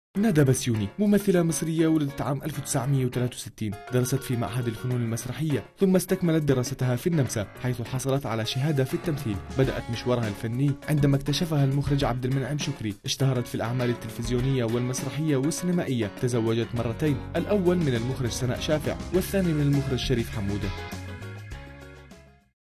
الممثلة المصرية ندى بسيوني
ندى بسيوني ممثلة مصرية ولدت عام ١٩٦٣، درست في معهد الفنون المسرحية ثم استكملت دراستها في النمسا حيث حصلت على شهادة في التمثيل، بدأت مشوارها الفني فندما اكتشفها المخرج عبد المنعم شكري، اشتهرت في الاعمال التلفزيونية المسرحية والسينمائية، تزوجت مرتين الاول من المخرج سناء شافع والثاني المخرج شريف حمودة.